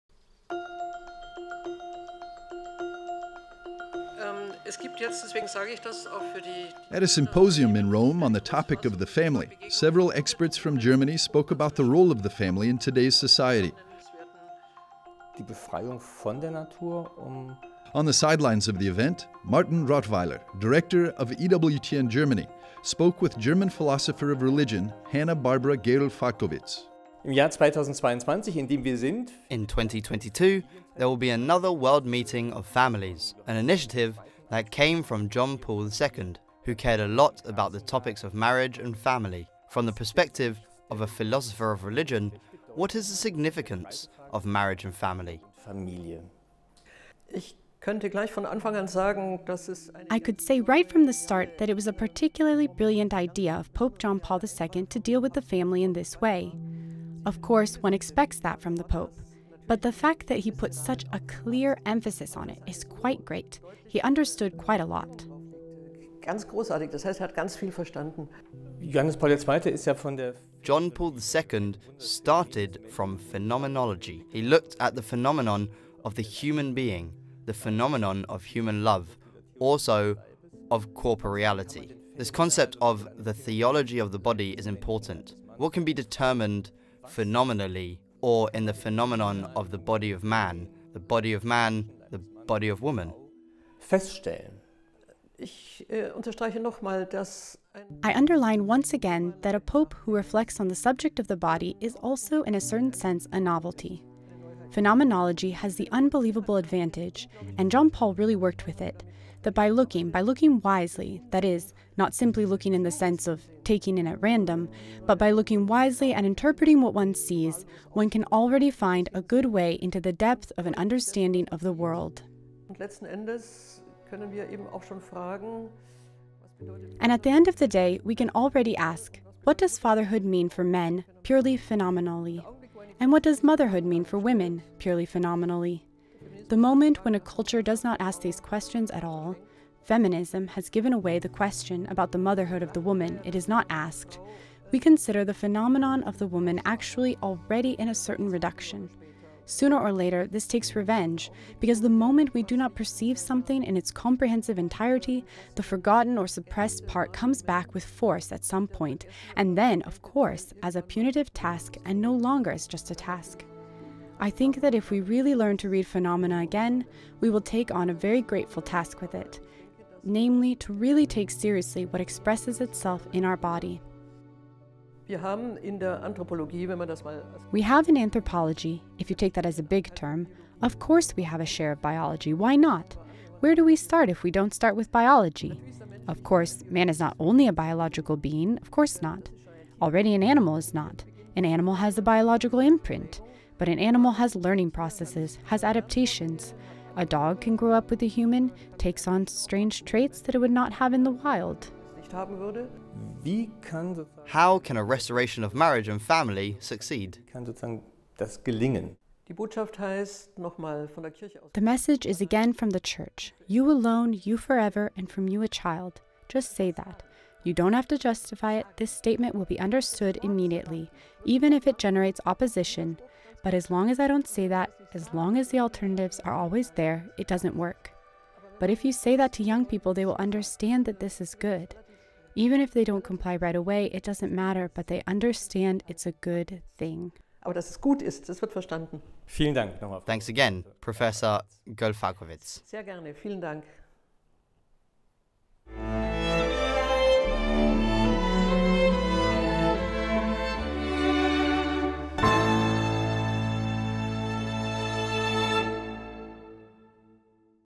Marriage and Family Through the Lens of Anthropology
At a symposium on the topic of the family in Rome, several experts from Germany spoke about the role of the family in today's society. On the sidelines of the event, Martin Rothweiler, director of EWTN Germany, spoke with German philosopher of religion Hanna-Barbara Gerl-Falkovitz.